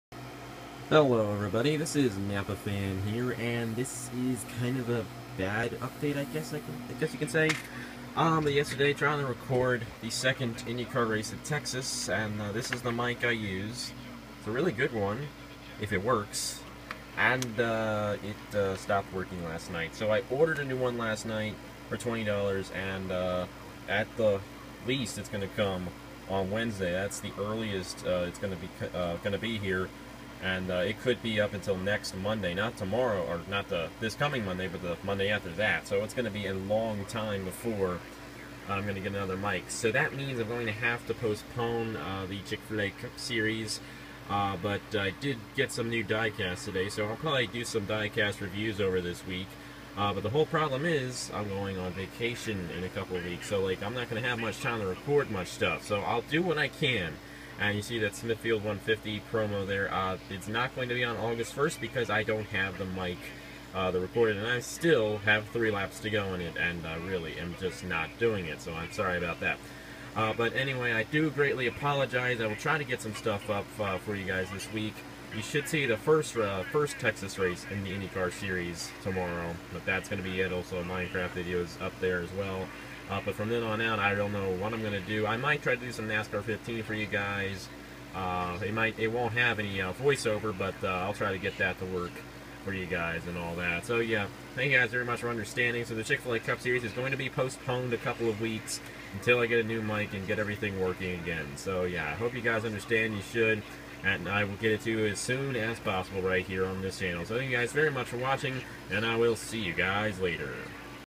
CFACS will be postponed! Watch for details.
Sorry guys, but my mic is broken. I'll get Season 3 of the CFACS up as soon as I can! Thanks for understanding!